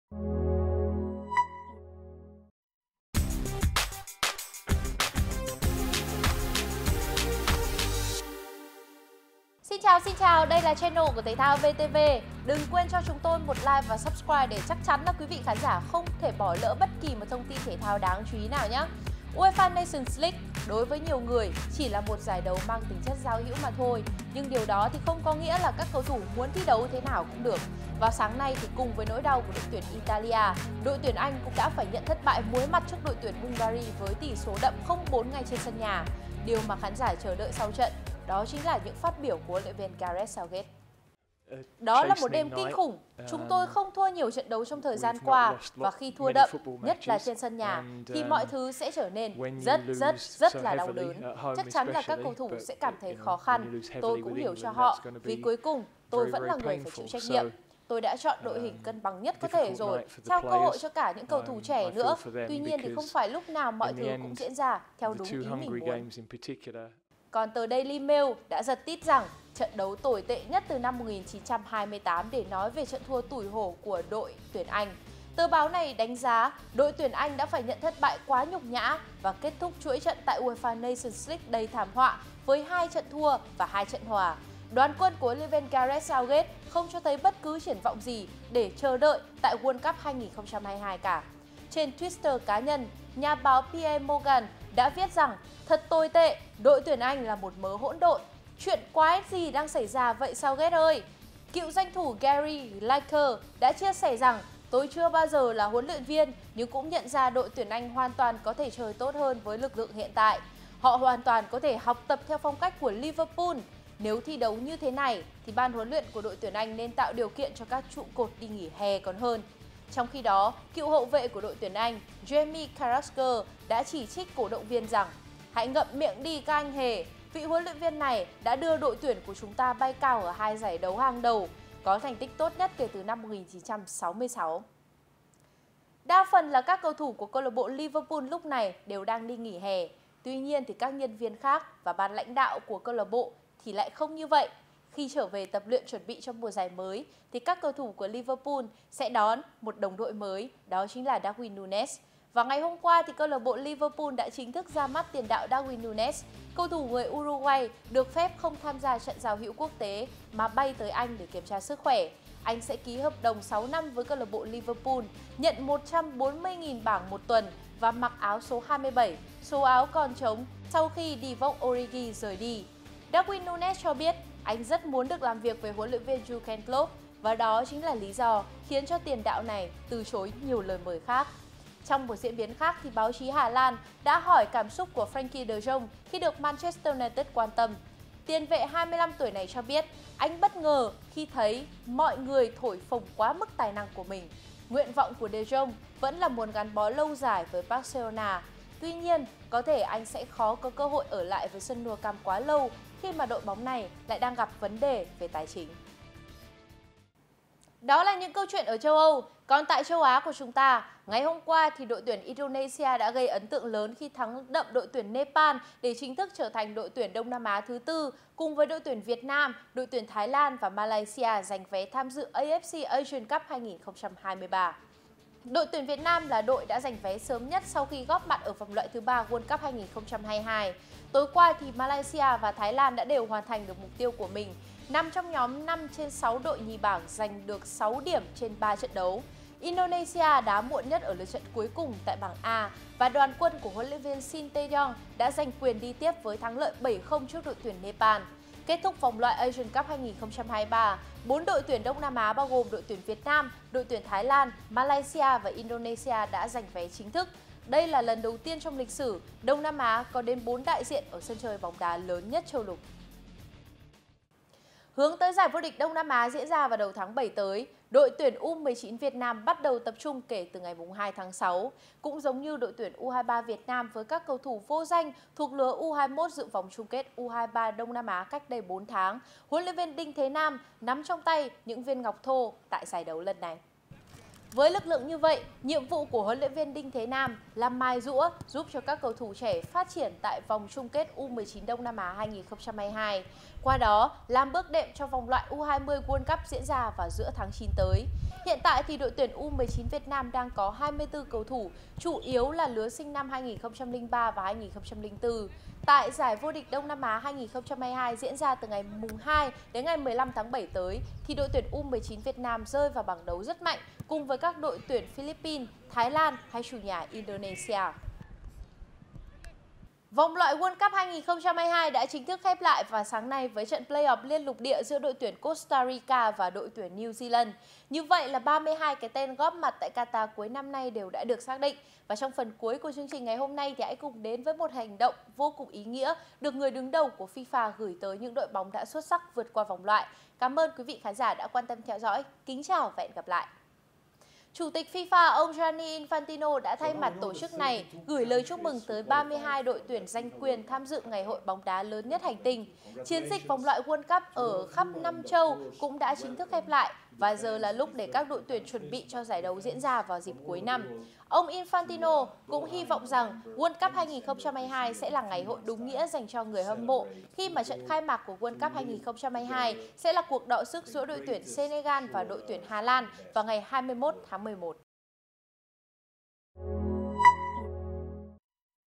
Tin nóng thể thao chiều 15/6 | ĐT Anh nhận "mưa" chỉ trích, những viên ngọc thô U19 Việt Nam
Chứng kiến thất bại thảm hại của đội nhà, dĩ nhiên là truyền thông Anh không thể không có những bài viết chỉ trích thầy trò HLV Southgate. Trong khi đó, cũng giống như U23 Việt Nam với các cầu thủ vô danh thuộc lứa U21 dự VCK U23 Đông Nam Á cách đây 4 tháng, HLV Đinh Thế Nam nắm trong tay những viên ngọc thô tại giải đấu này.